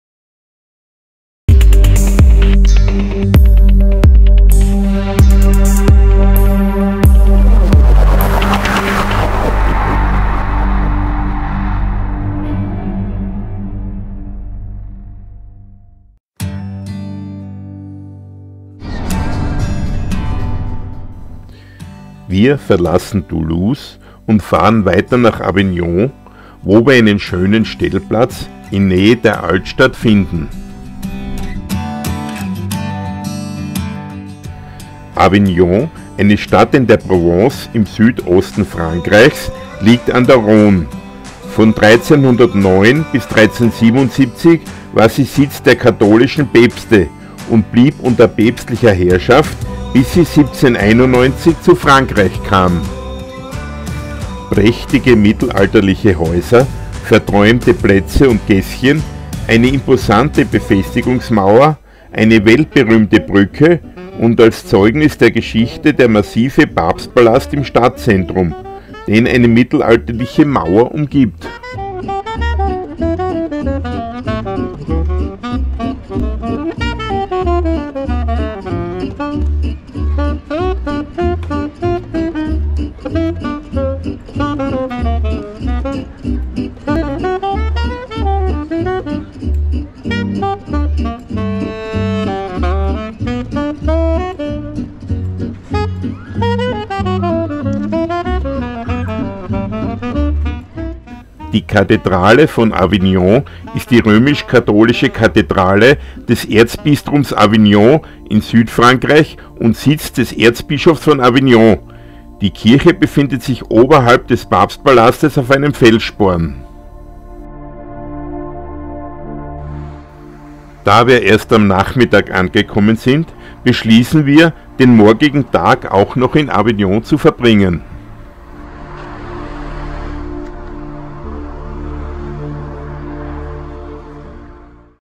Mit dem Wohnmobil 2022 durch Italien, Frankreich, Portugal und Spanien Tag 44: Avignon
44. Tag unserer fast siebenwöchige Tour mit dem Wohnmobil von September bis November 2022
Tag 44: Avignon